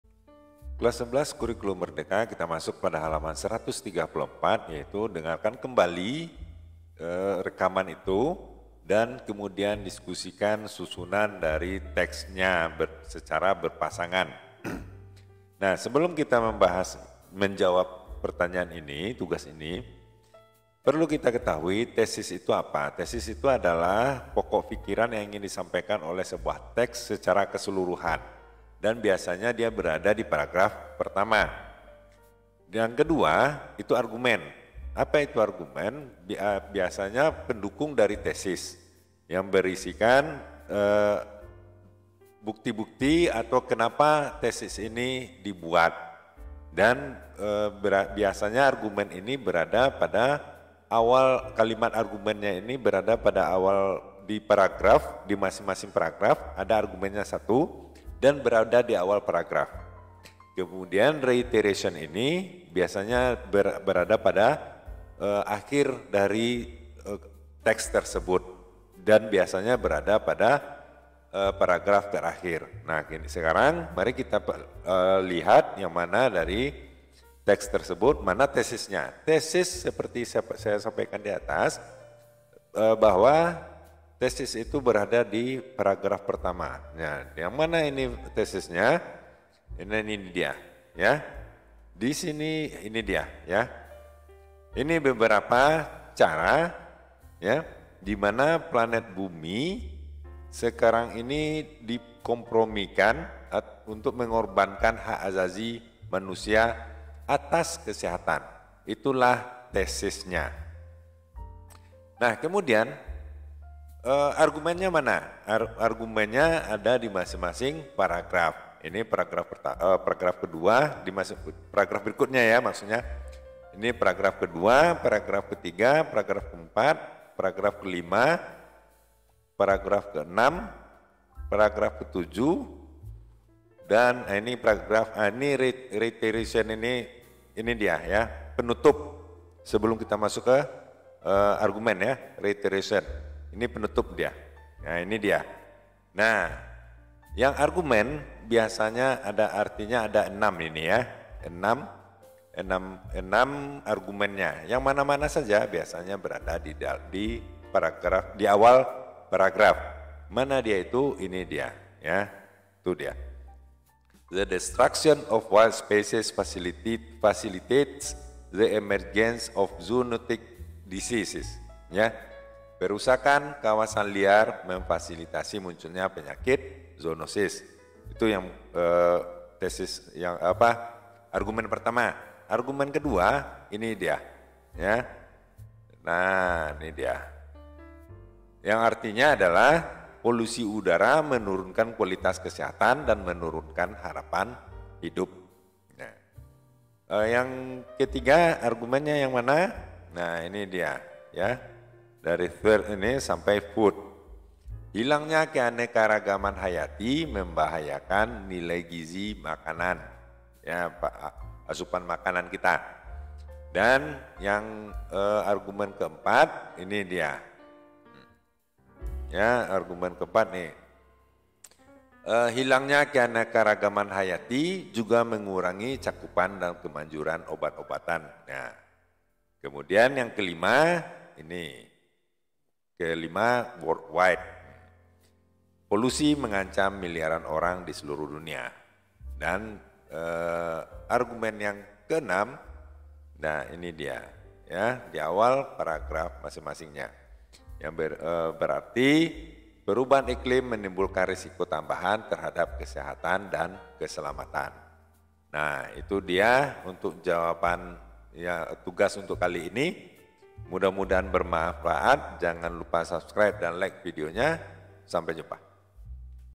Hal. 134 kelas 11 Terjemahan Buku Bahasa Inggris SMA MA Kurikulum Merdeka
Mengerjakan tugas mencari struktur teks dari Reasons Why a Healthy Environment should be a Human Right
terjemahin yuk
terjemahinyuk
terjemahkan yuk
Kelas VIII
English for Nusantara
Tingkat Lanjut
kunci jawaban
belajar bahasa inggris
pelajaran
halaman